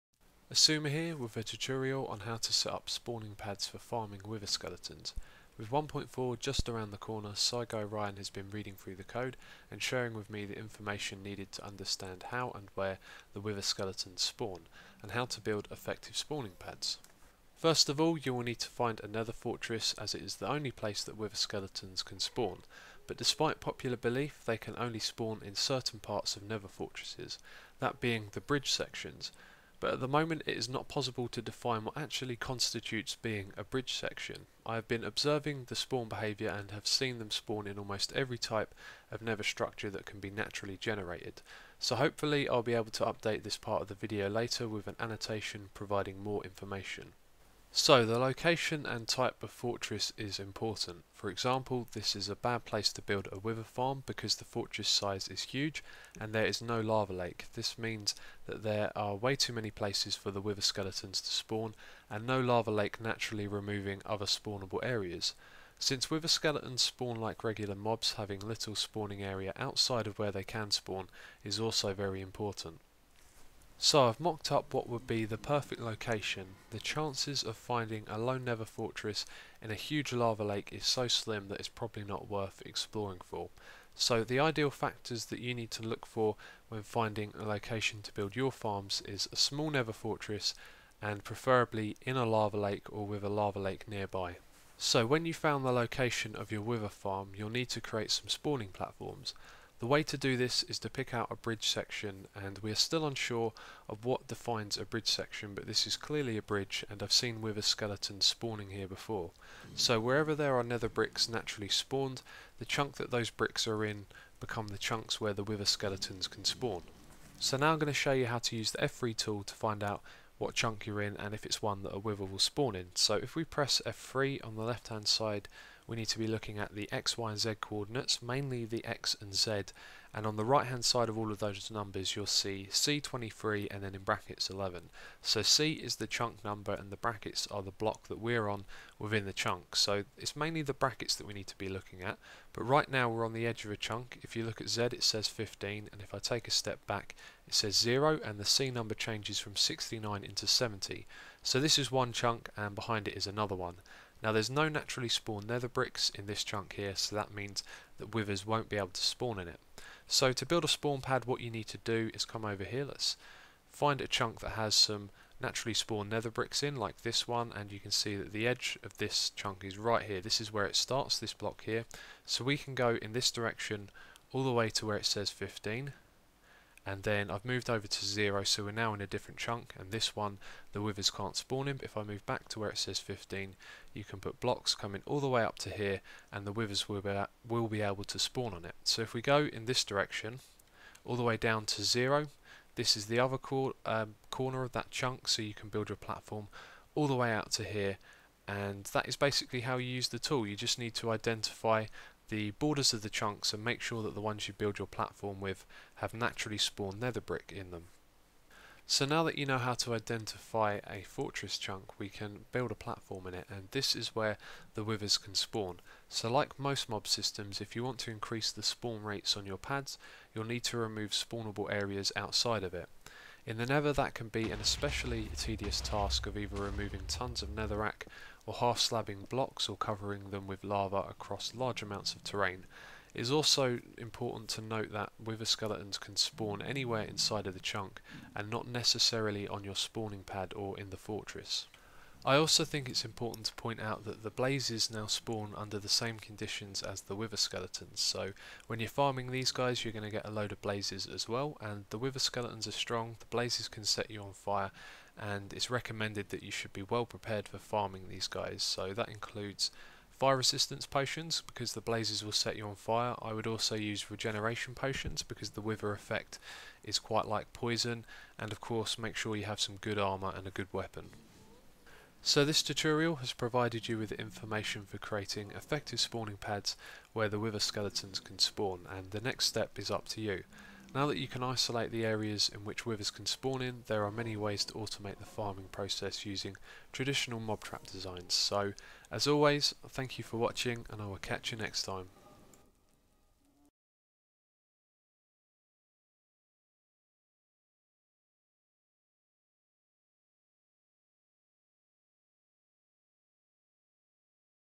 Wither Skeleton Farming Tutorial
| Get notified of all my uploads

This tutorial will go over the basics of how wither skeletons spawn in the nether so you can build spawn pads in the correct locations.